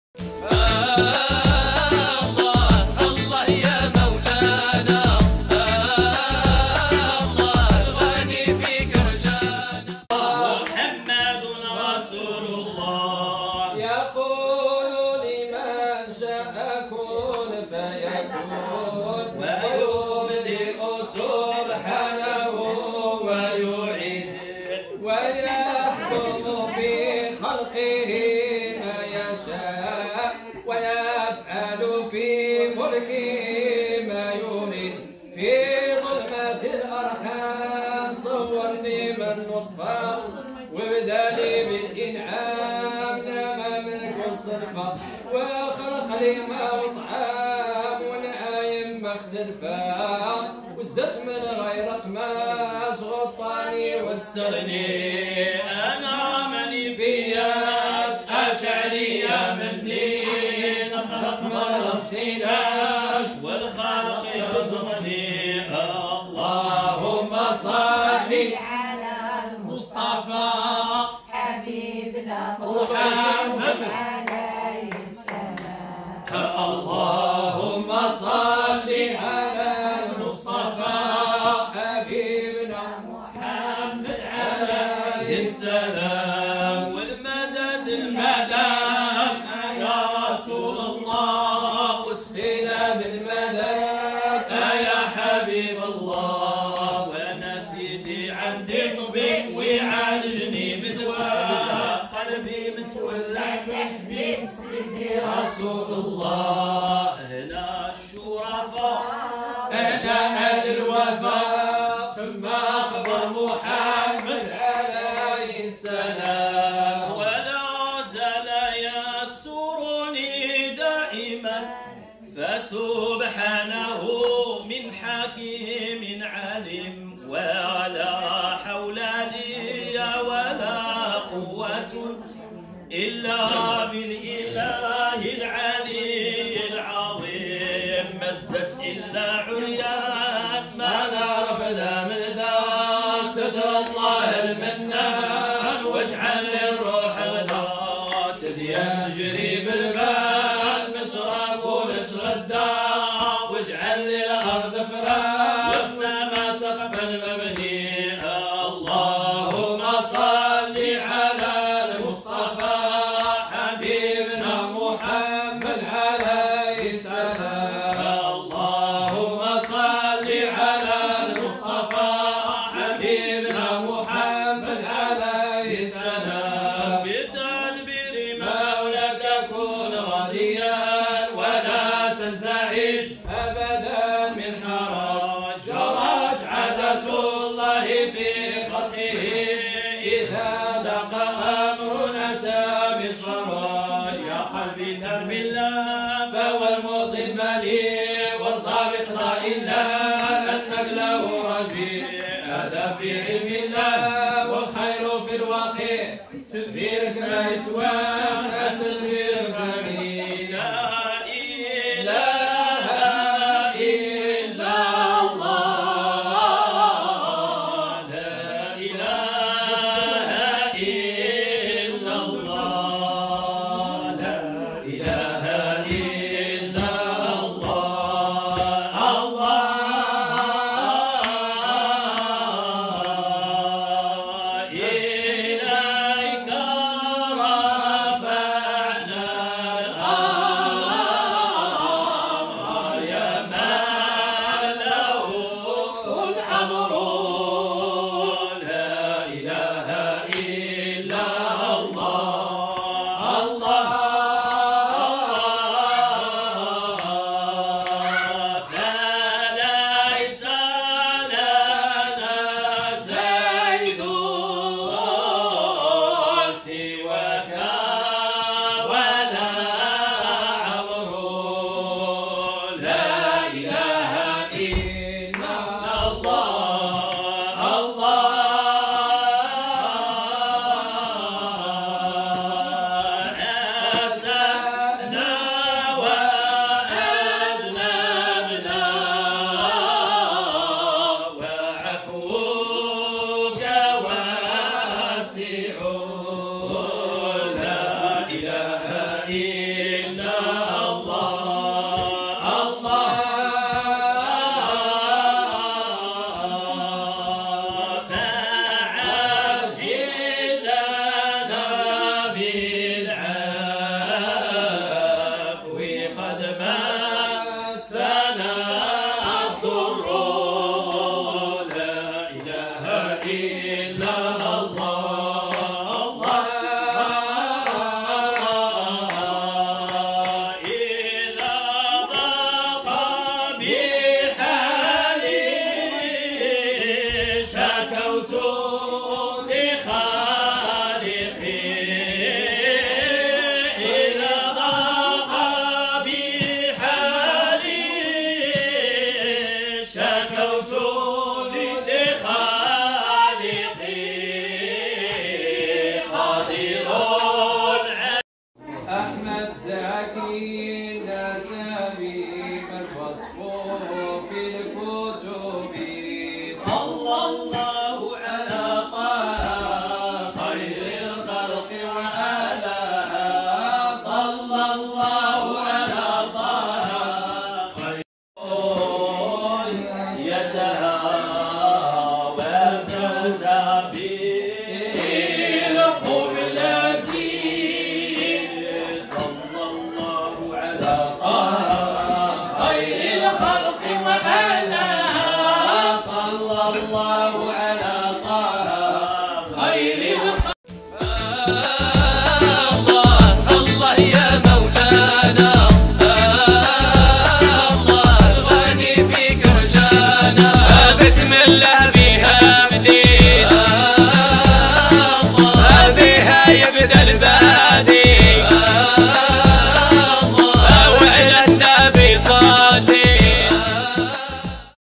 امداح نبوية في مناسبة حزينة 2021
wafae_ yousra_oumaima_jamila_leila_2021

اللهم إن عبادك في ذمتك وحبل جوارك، فقهم فتنة القبر، وعذاب النار، وأنت أهل الوفاء والحق، فاغفر لهم وارحمهم، إنك أنت الغفور الرحيم.

• اللهمّ ادخلهم الجنّة من غير مناقشة حساب، ولا سابقة عذاب.

• اللهمّ آنسهم في وحدتهم، وفي وحشتهم، وفي غربتهم.